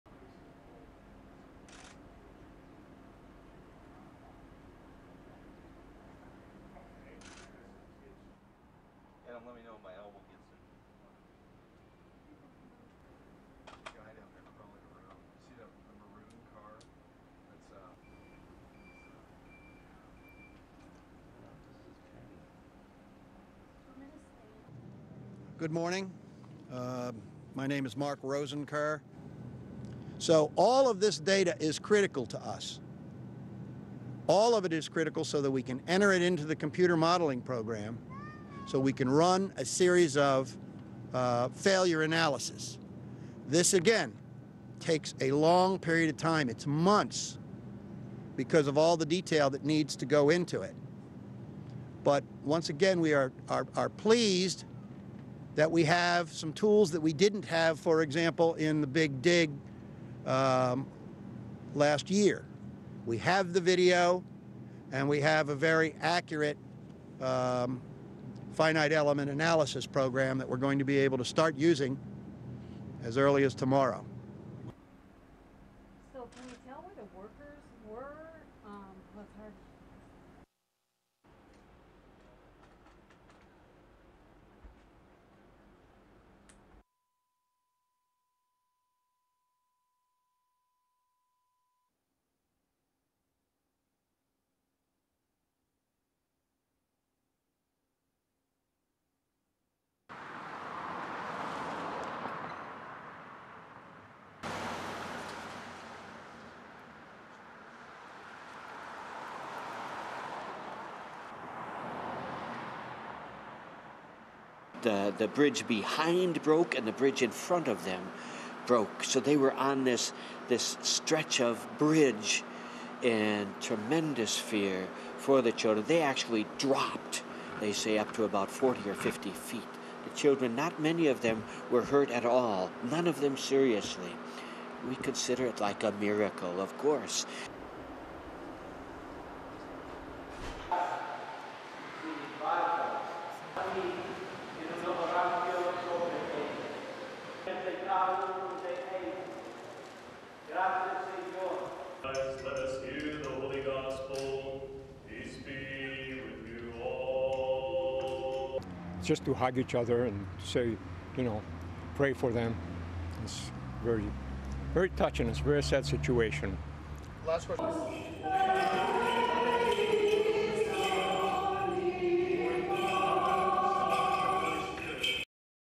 Recovery efforts continue at site of collapsed bridge, church services
Minneapolis
1. Workers making their way down to the collapsed bridge
2. Various of cars in the wreckage
3. Roof of submerged car
4. National Transportation Safety Board (NTSB) official Mark Rosenker arriving at news conference
5. SOUNDBITE: (English) Mark Rosenker, Chairman of National Transportation Safety Board (NTSB):
"So, all of this data is critical to us. All of it is critical so we can enter it into the computer modelling programme so we can run a series of failure analysis.  This again, takes a long period of time, it's months because of all the detail that needs to go into it.  But once again, we are pleased that we have some tools that we didn't have for example in the big dig (referring to the tunnel collapse in Boston) last year.  We have the video and we have a very accurate finite element analysis programme that we're going to be able to start using as early as tomorrow."
6. Cars in wreckage
7. Portion of collapsed bridge
8. Workers navigating docking boat
9. Searchers in small boat on shoreline
10. Wide exterior of Holy Rosary church
11. Church sign
12. Cross on top of church
13. SOUNDBITE: (English) Father Jim Varnett, Priest at the Holy Rosary church:
"The bridge behind them broke and the bridge in front of them broke.  So they were on this stretch of bridge and tremendous fear for the children.  They actually dropped they say up to about forty or fifty feet. The children, not many of them were hurt at all, none of them seriously.  We consider it like a miracle of course."
14. School bus behind partially crushed truck
15. Various of children at church service
16. Church attendees
St Paul, Minnesota
17. Wide of service at St George
18. SOUNDBITE: (English) Chris Kontenakos, service attendee:
"Just to hug each other and pray for them it's very touching.   It's a very sad situation."
19. Priest during service
STORYLINE:
The National Transportation Safety Board (NTSB) investigation into the deadly Minnesota bridge collapse continued on Sunday with investigators warning it could take "months" to complete the analysis.
On Sunday, the NTSB confirmed it had been working with Progressive Contractors Inc. the company that was working on the eight-lane span when it collapsed into the Mississippi River, taking 18 Progressive employees with it.
One of the workers is feared dead.
The contract required the company to repair the bridge deck, replace the concrete surface and expansion joints and work on the anti-icing system.
NTSB said it had interviewed company officials and workers and was analysing construction and maintenance documents.
The roadway project of Progressive Contractors Inc. is one of a range of issues being investigated including aging steel, deteriorating welds, vibration from adjacent train tracks and even the corrosive effects of bird droppings.
NTSB officials say they will aim to preserve as much of the scene to get data on exactly what the situation was at the moment of failure and will be looking at a wide range of issues from temperature that day, number of cars on the bridge, and type of equipment that may have been on the bridge.
Mark Rosenker, the Chairman of NTSB said "all of this data is critical to us."
Meanwhile survivors and the victims were remembered in several church services on Sunday.
At the Holy Rosary church a service was held to celebrate the survival of children who were in the school bus when the bridge collapsed.
Father Jim Varnett described the outcome as a "miracle."
The mood was more sombre in the city of St Paul just on the other side of the bridge, where they were mourning one of the victims.